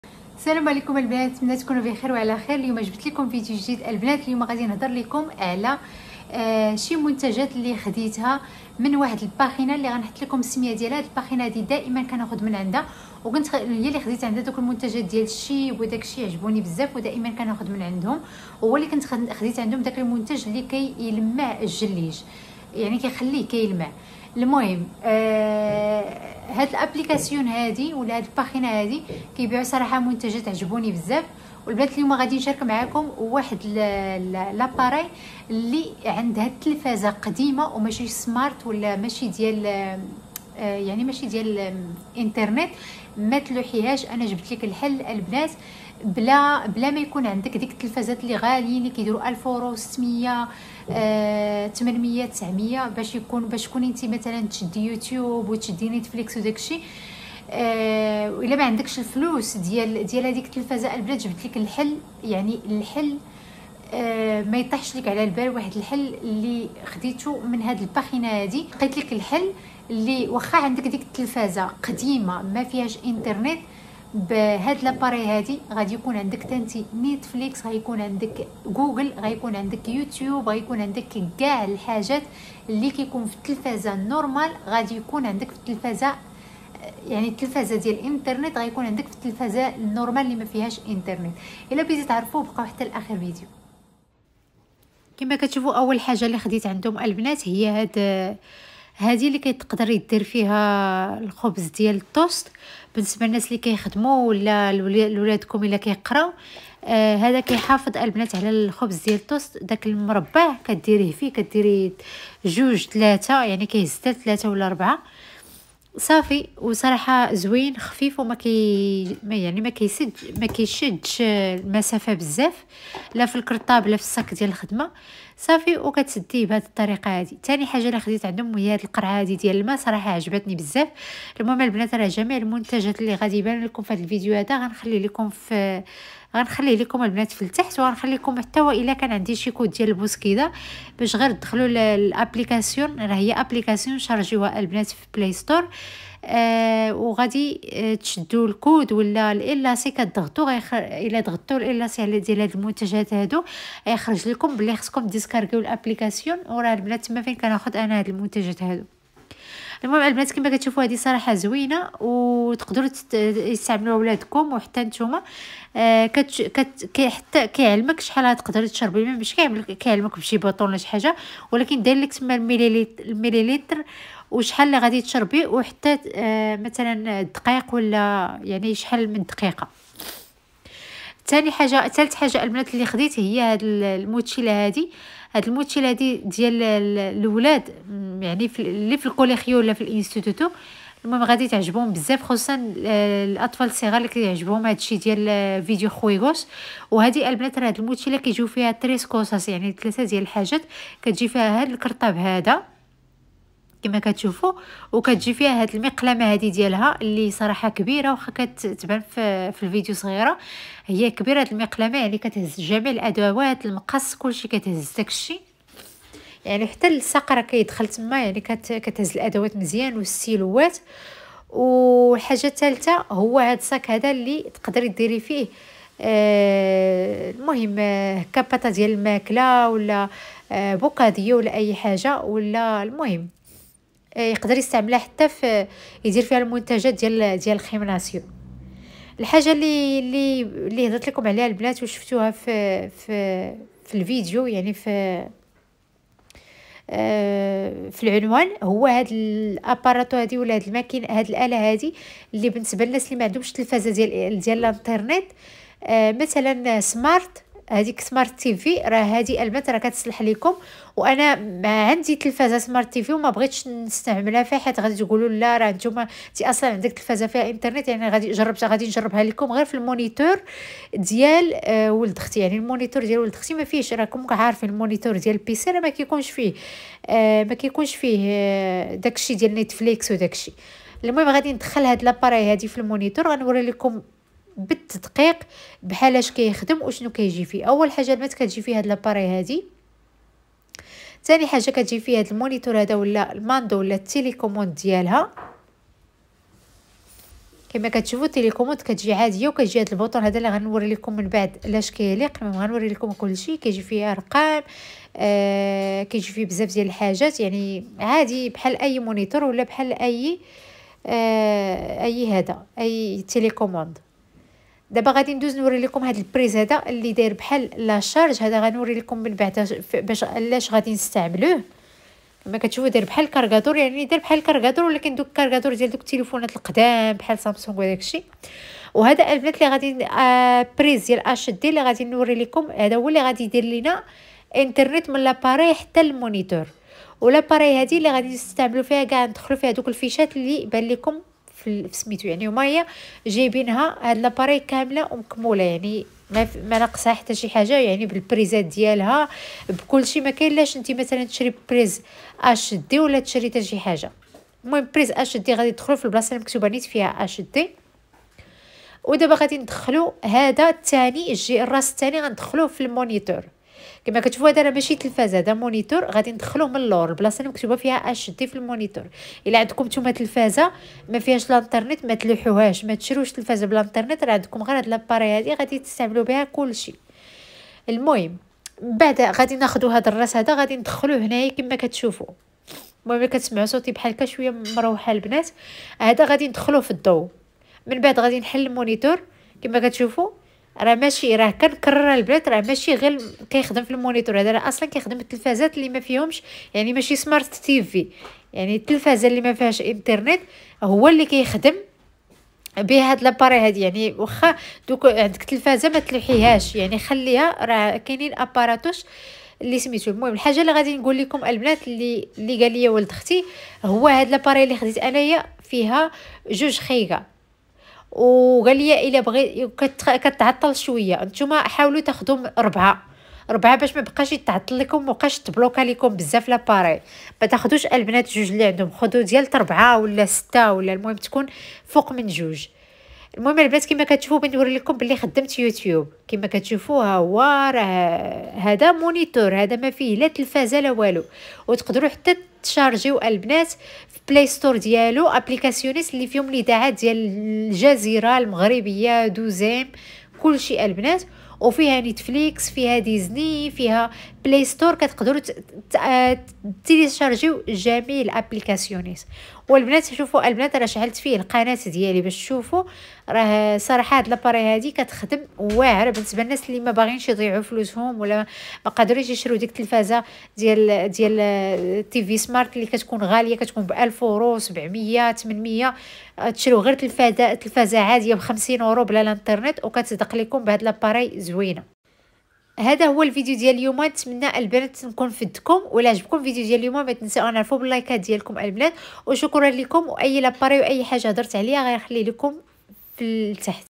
واخيراً حل لتلفزات القدام بلا إنترنيت 🎊 تبرعي بقنوات و نيتفليكس بلا ما تشري تليفيزيون غالي/ ANDROID
--【Code[HAS1] to get 10 EUR OFF on orders over 10.1 EUR】
--【Code[HAS2] to get 16 EUR OFF on orders over 80.1 EUR】